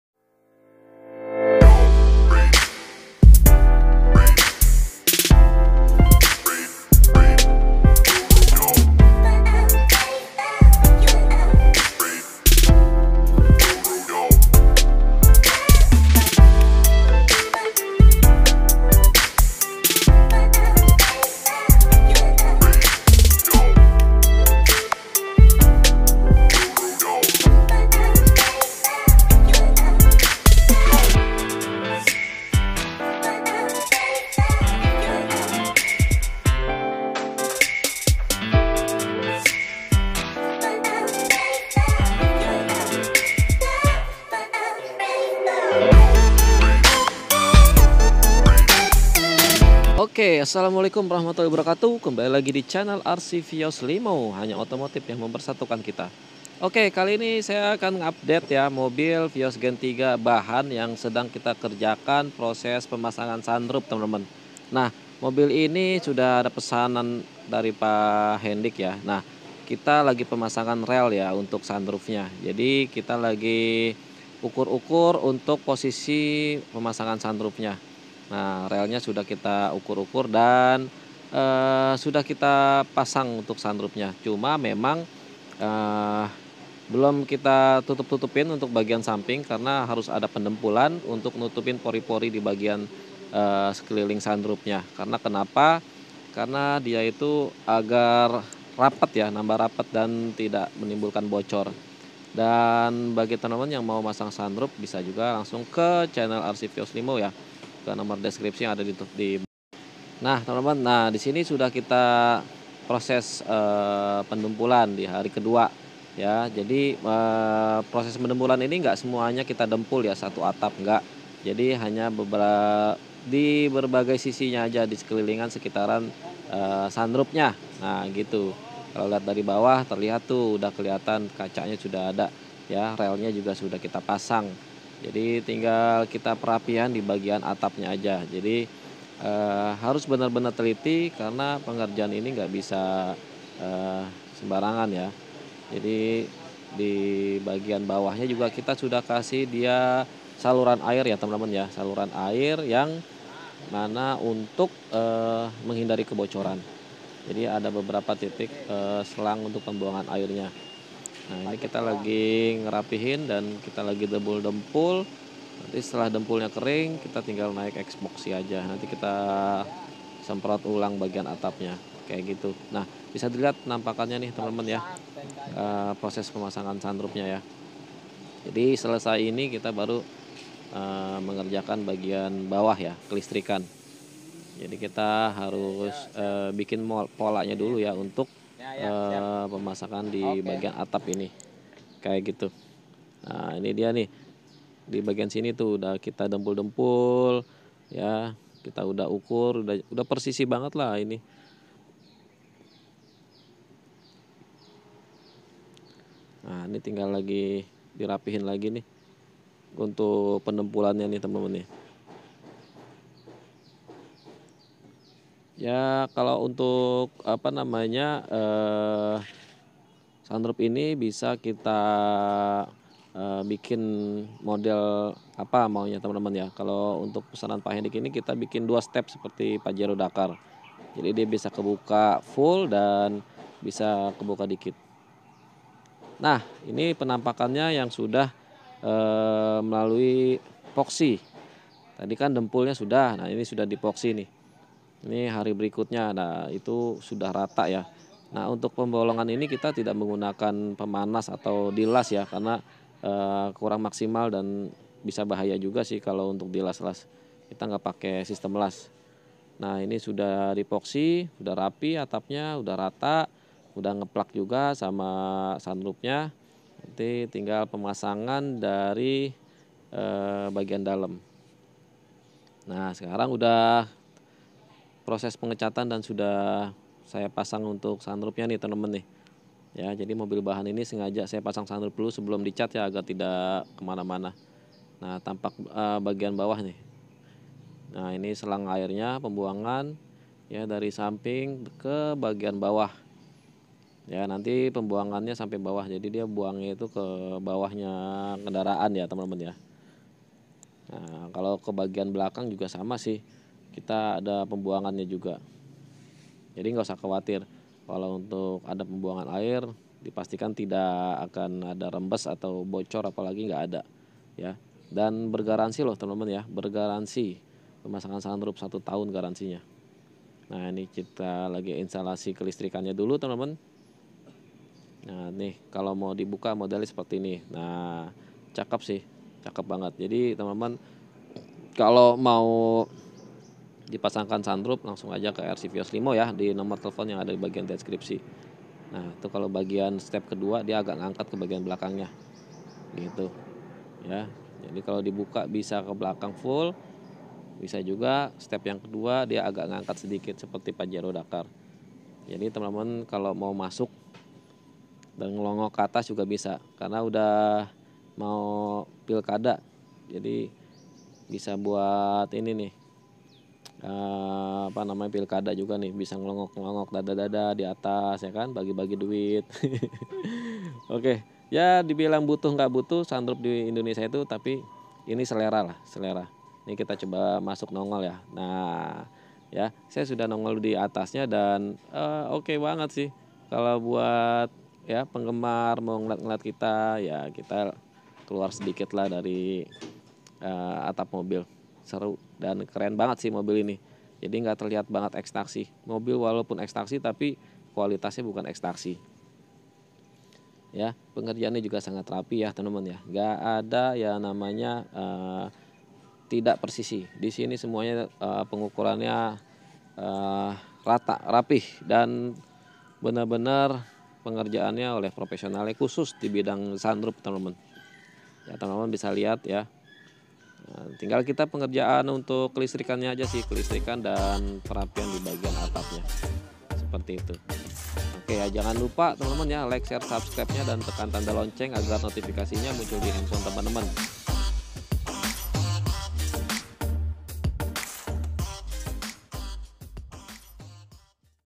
Proses Pemasangan Sunroof Vios gen 3 ex Bluebird / Project bahan Rcvios Part 1
Proses Pemasangan Sunroof Vios all new limo gen 3

Pengerjaan selama 7 hari dan dikerjakan oleh yang di bidangnya, jd tidak bisa sembarangan asal pasang, bisa-bisa bocor kalo bukan ahlinya

Pemasangan Di Rcvios bergaransi ya, dan insyaAlloh Qualitas Bagus